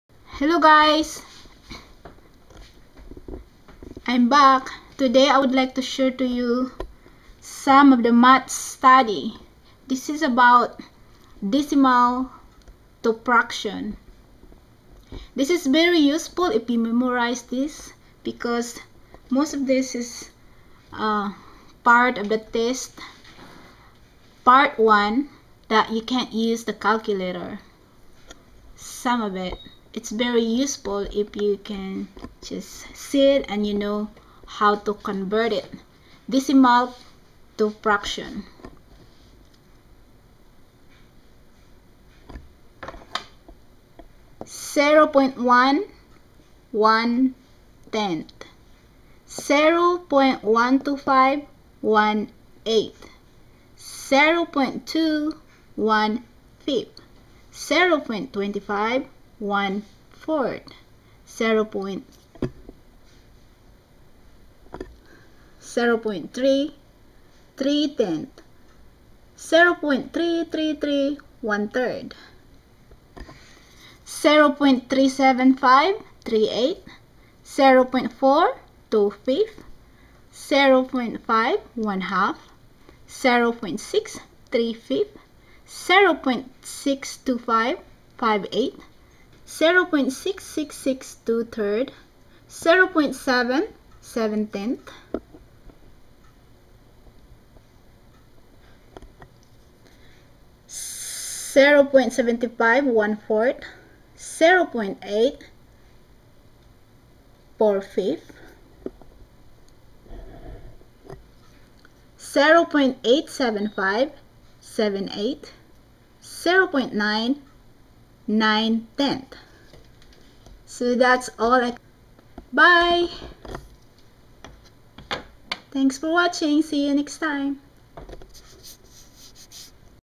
2016 GED part 1 of Math test decimal to fraction.
Must memorize these decimal to fraction for some of these are in part 1 math test, that you can't use  calculator.